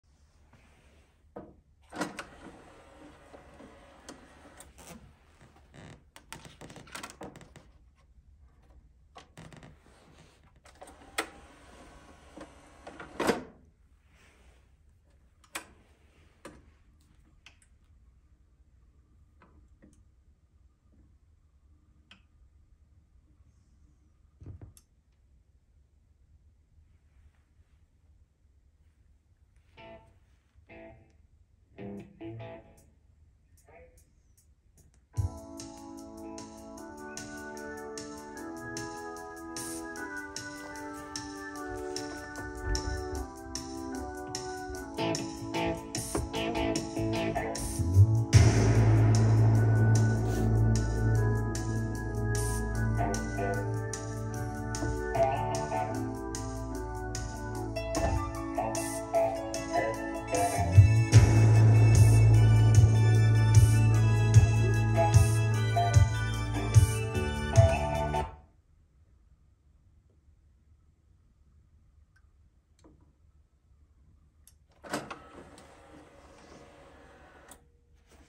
AIWA AP-D50E turntable with drawer
AIWA AP-D50E turntable with drawer. You don't have to open the top cover to loading the vinyl this turntable have a special mechanism which work like a tray in CD player.